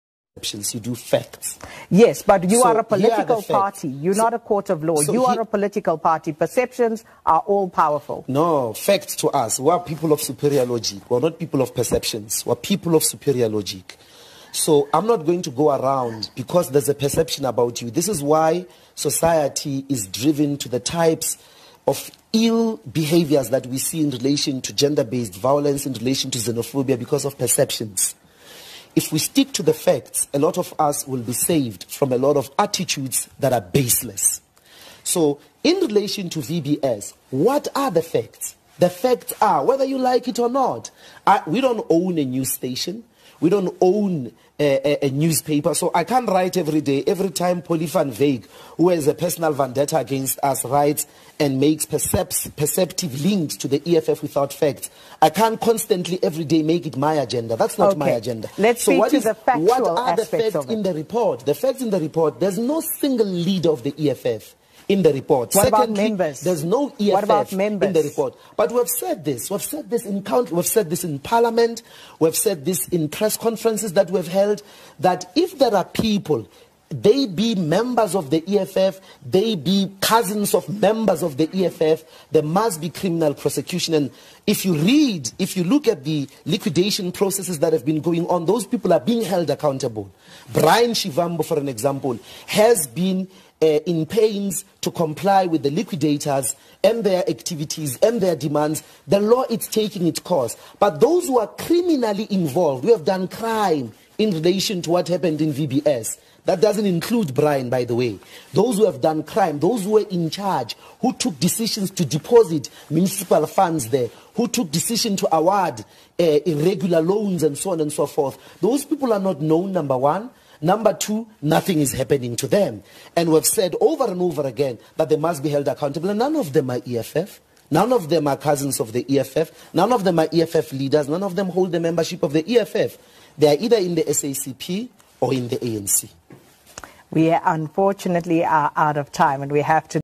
EFF Dr. Mbuyiseni Ndlozi leaves the anchor in stitches with his answer on VBS Scandal
On SABC interview, Economic Freedom Fighters MP Dr. Mbuyiseni Ndlozi answers questions relating to VBS looting.